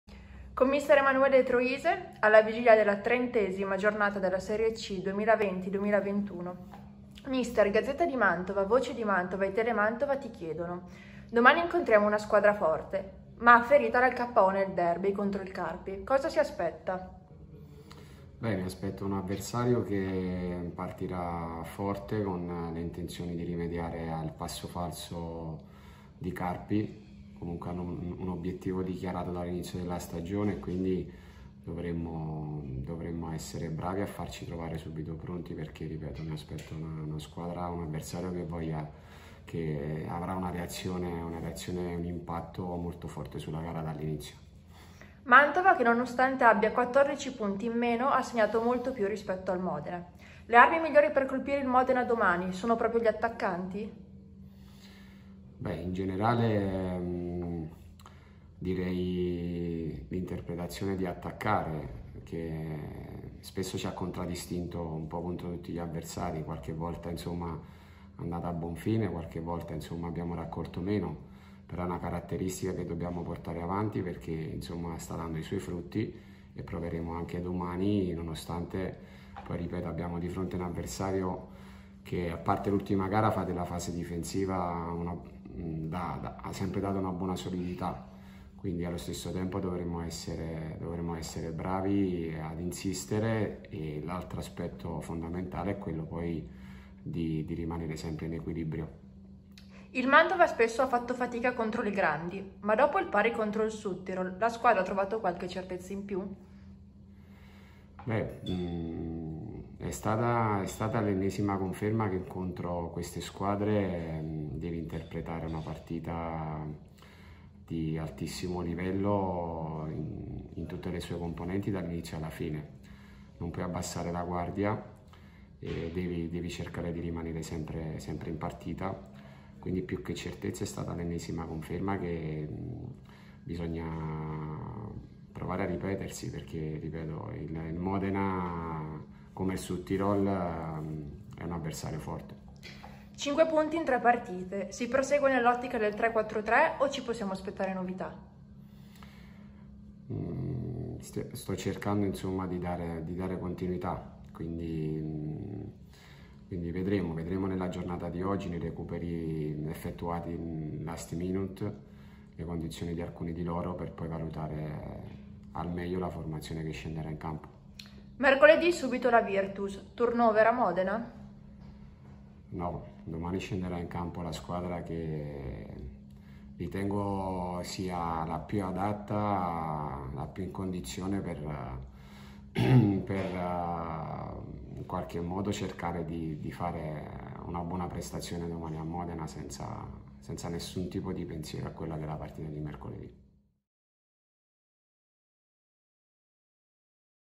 Modena-Mantova, Troise: "Mi aspetto un Modena agguerrito. Servirà una prestazione di alto livello"
📹 Le principali dichiarazioni dell'allenatore biancorosso Emanuele Troise, rilasciate alla vigilia di Modena-Mantova, 30a giornata della Serie C 2020/21 💪⚪🔴